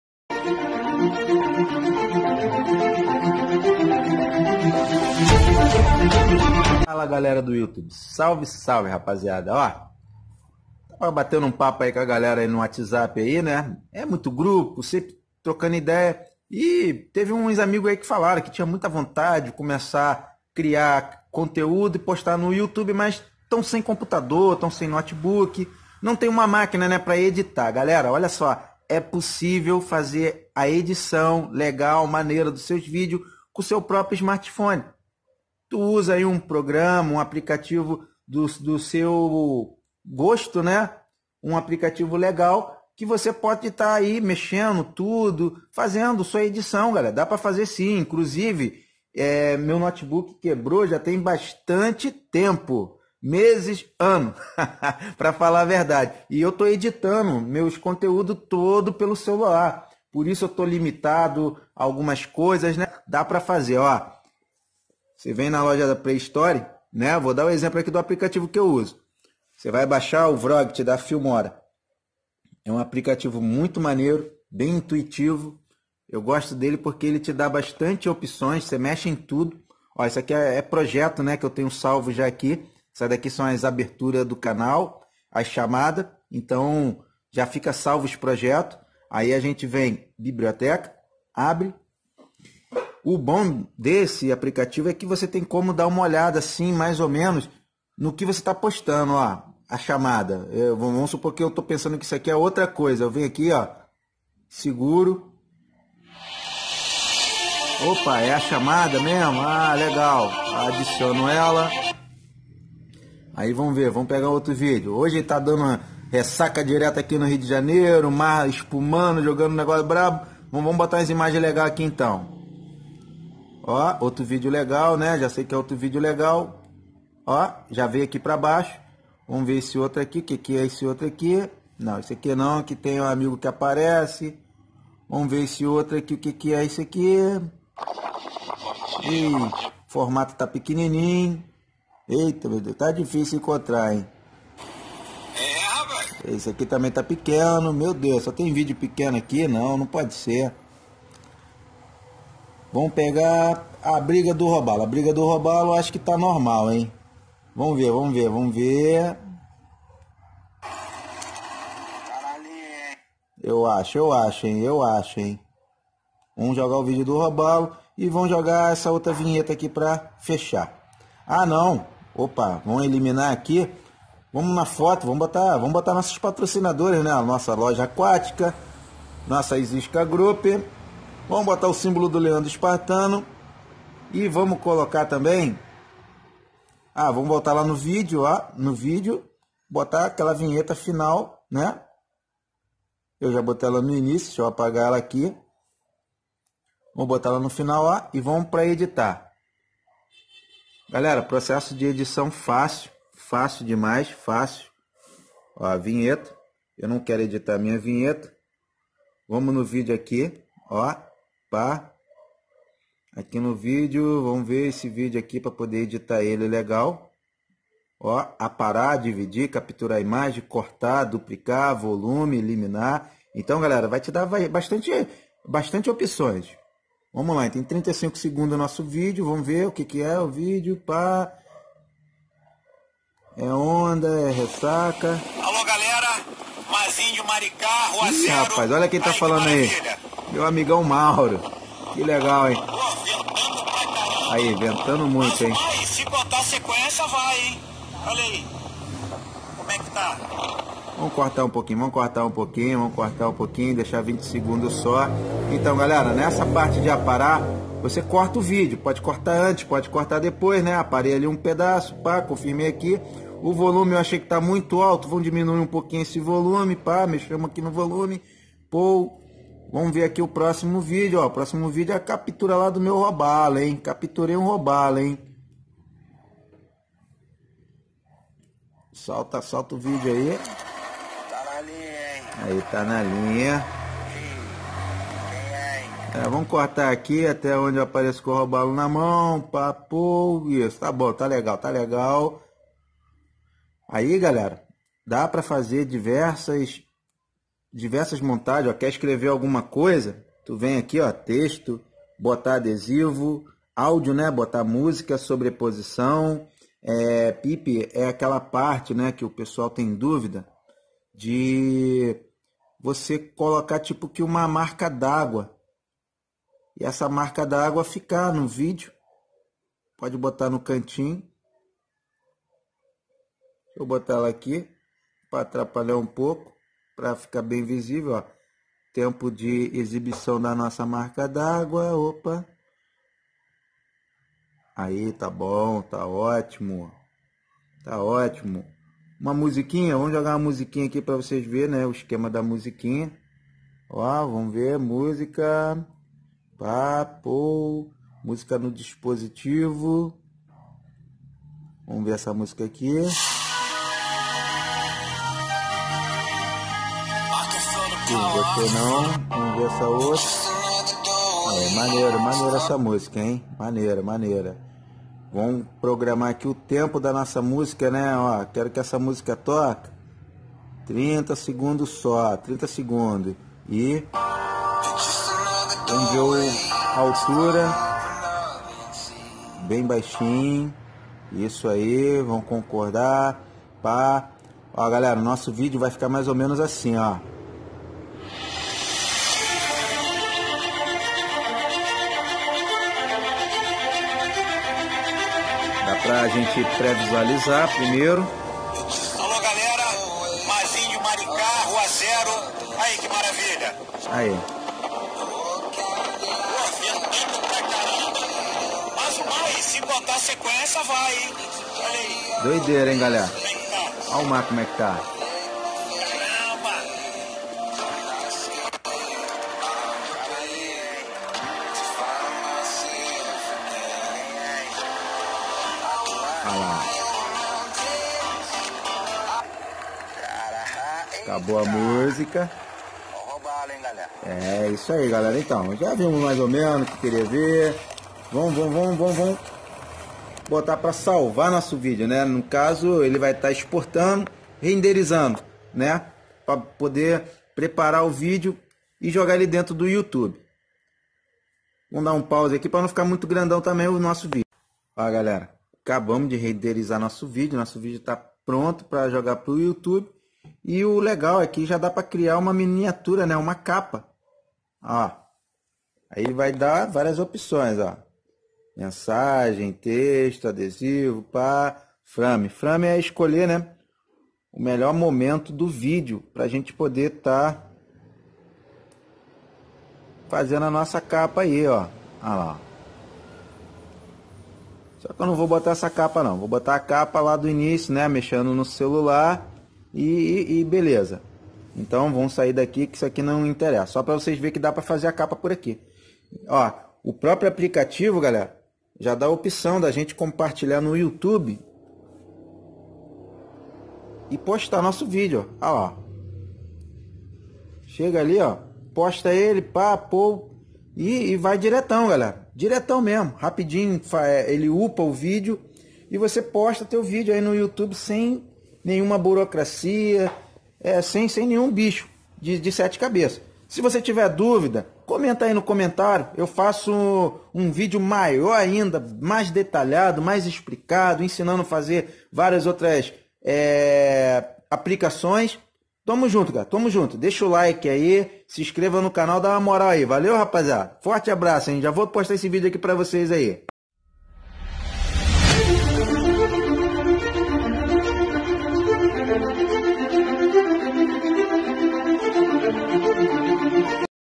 É possível editar e postar vídeo no YouTube pelo celular ?
Fala galera do YouTube
Você está querendo postar seus vídeos ou começar no ramo de criadores de conteúdo para o YouTube,mas está sem computador ou notebook ?
Então seus problemas acabaram,eu também estou nessa mesma situação e meus vídeos não param kkkk
Eu uso o meu próprio celular para editar e postar todos os meus conteúdos na plataforma.
Neste vídeo explicarei o processo básico,mas qualquer dúvida é só comentar.
E qualquer coisa explico melhor o processo em outro vídeo voltado pra ajudar a galera.

E se inscrevam para dar uma força para o canal 👍🏼👍🏼